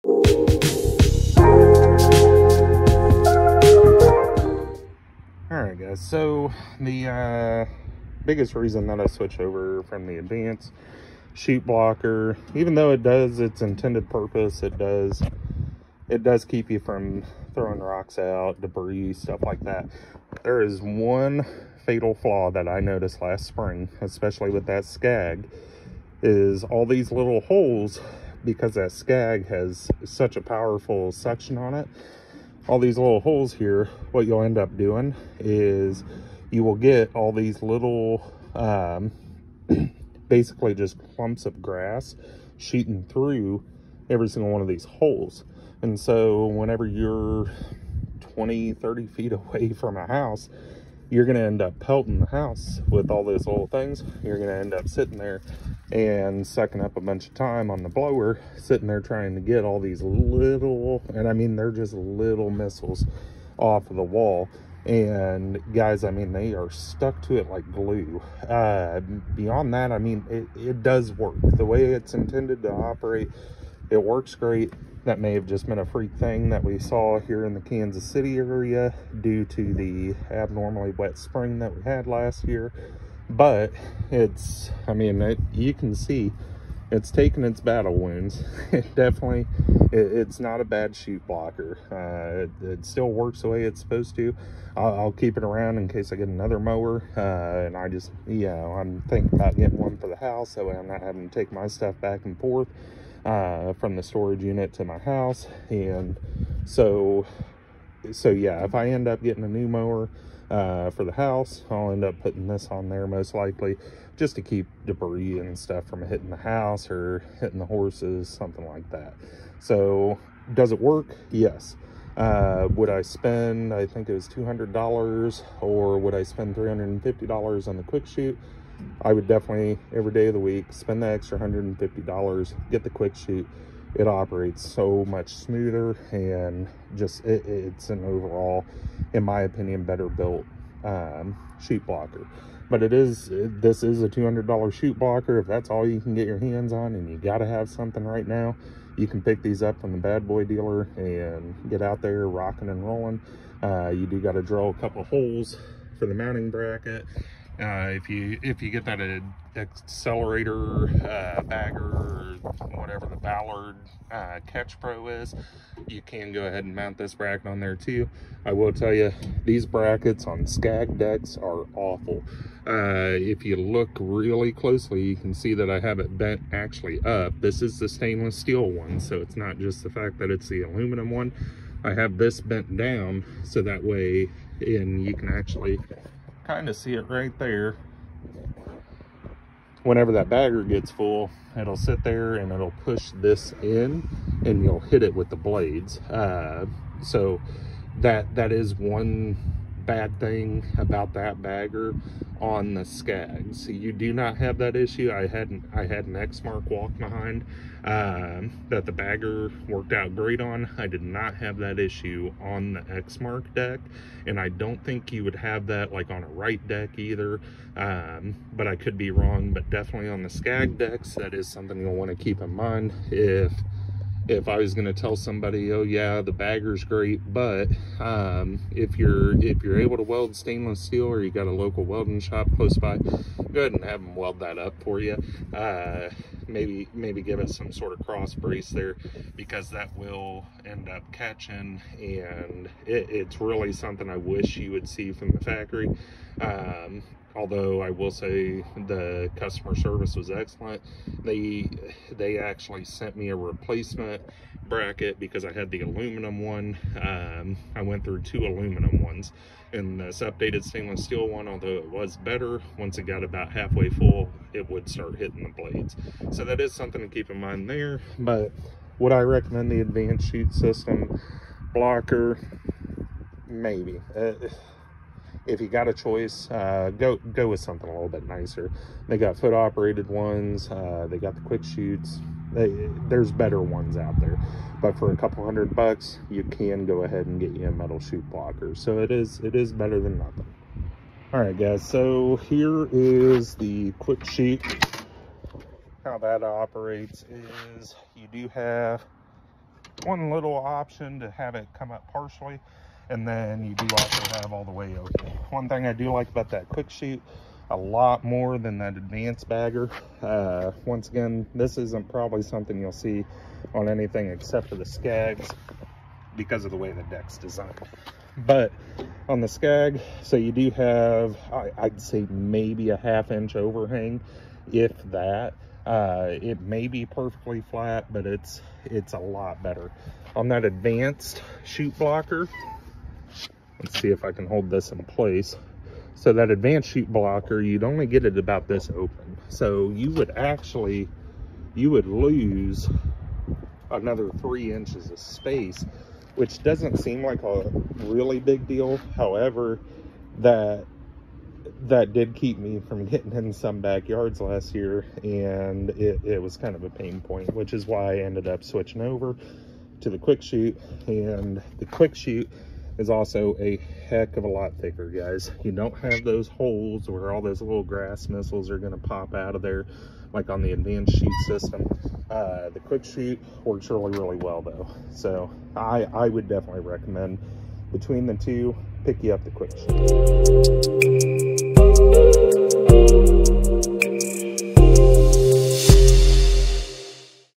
Advanced chute blocker vs Quickchute review!
Today we will be reviewing the advanced chute blocker vs the quickchute. I dive into what I’ve liked about both blockers so far and give my opinion about both of them as well as a review of the Ballard aluminum bagger bracket for the scag velocity decks.